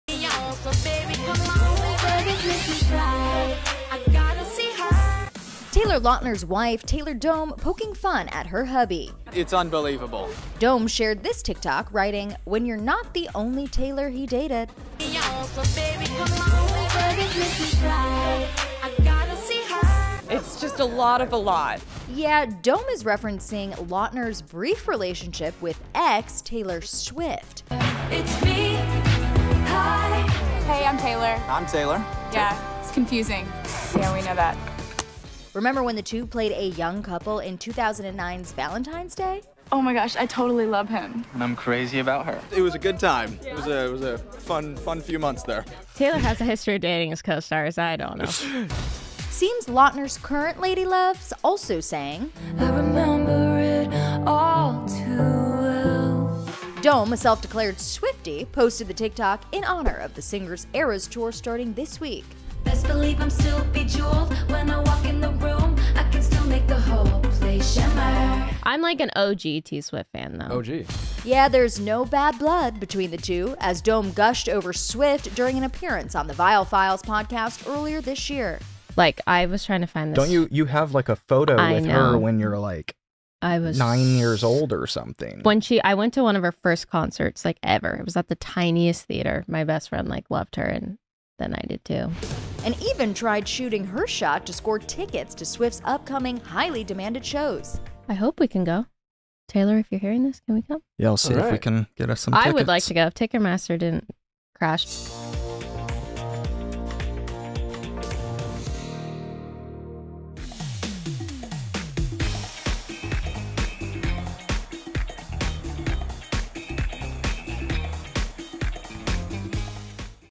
Taylor Lautner's Wife Pokes Fun at Him Previously Dating Taylor Swift
Taylor Lautner's wife, Taylor Dome, pokes fun at his dating history. Dome teased her hubby in a new TikTok, calling out his romances with women sharing the same name as him, including Taylor Swift.